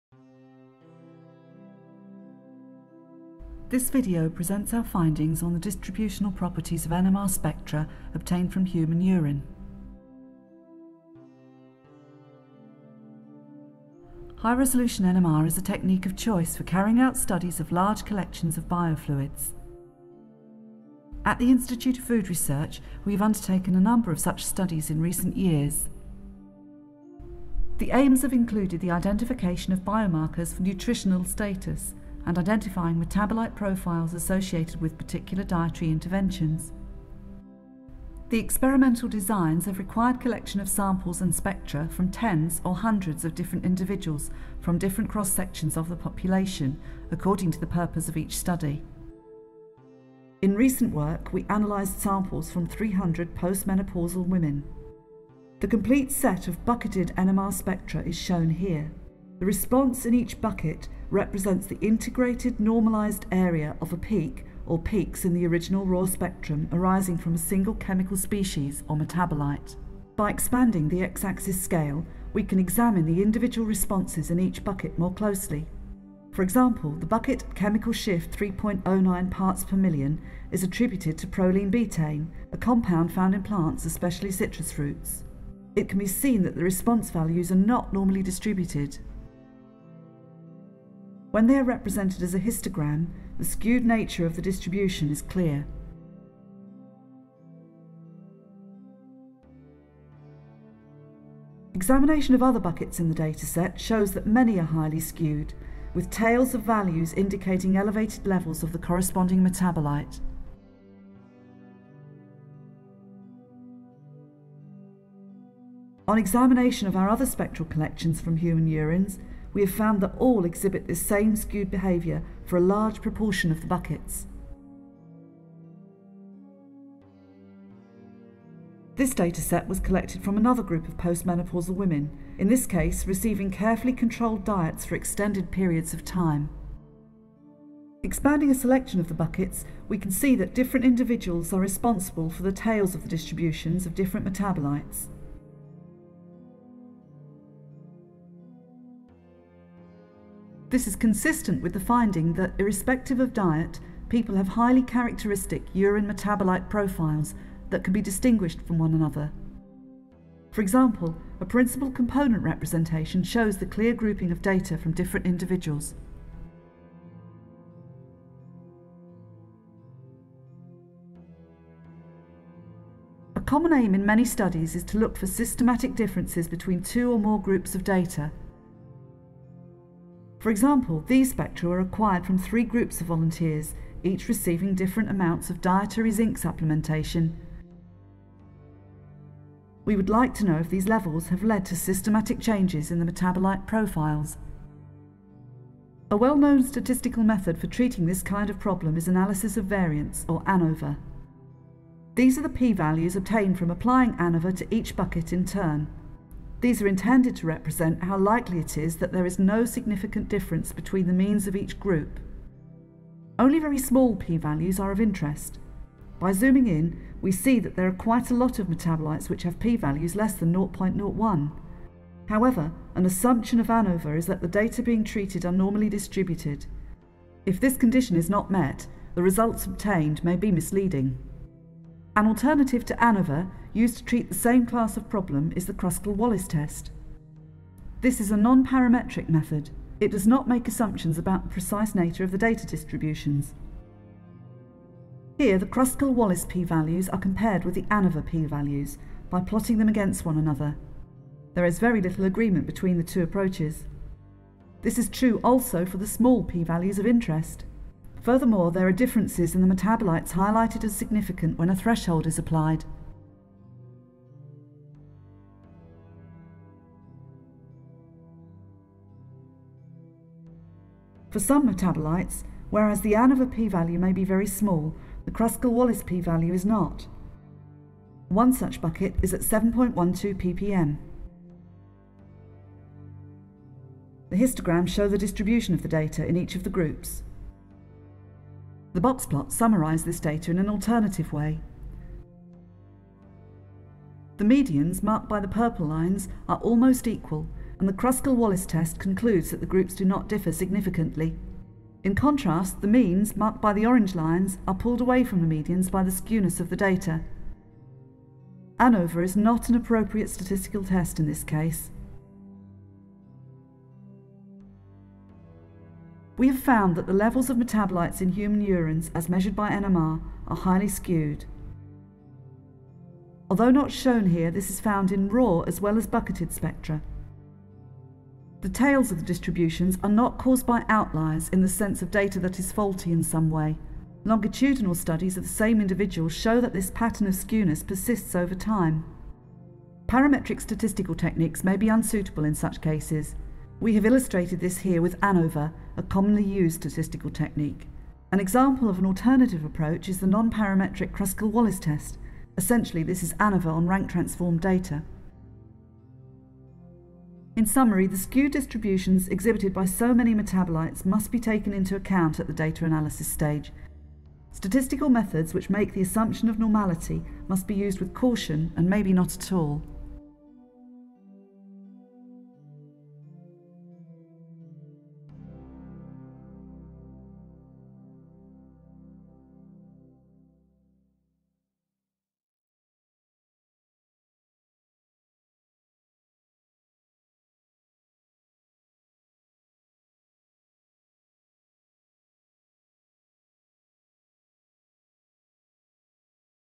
1H NMR Metabolomics Study of Human Urine
A presentation of recent work from the Analytical Sciences Unit at the Institute of Food Research, Norwich, UK.

This video is a shortened version of a presentation given at "Advances in Metabolic Profiling 2011".